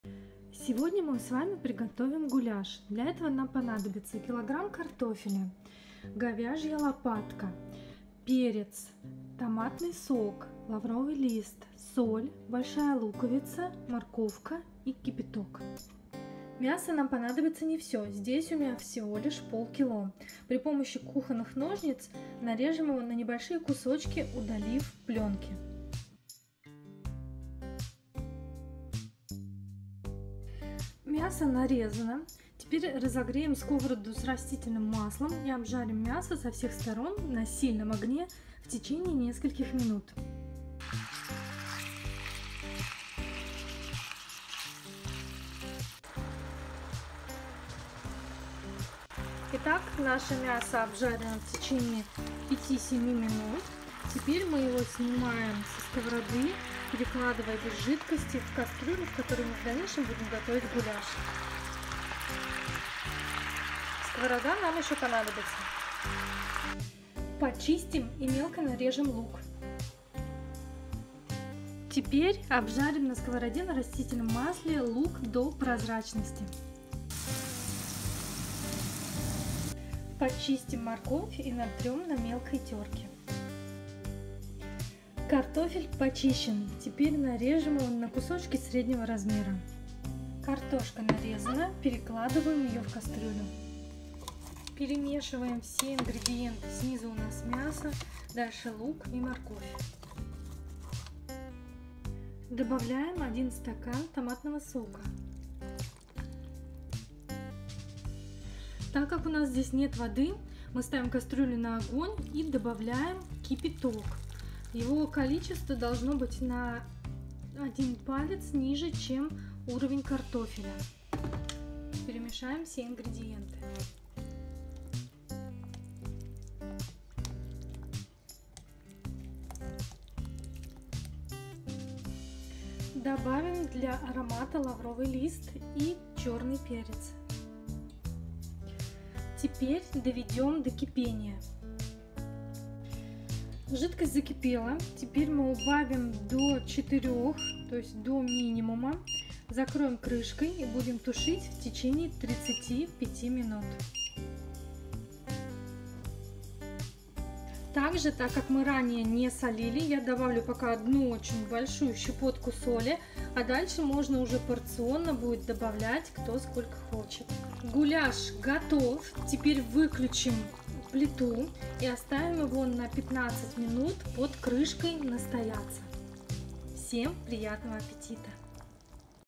Гуляш | ПРОСТОЙ РЕЦЕПТ
ИНГРЕДИЕНТЫ:
картофель - 1 кг
говядина (лопатка) - 500 г
томатный сок - 1 стакан (300 мл)
перец
соль - щепотка
лавровый лист
кипяток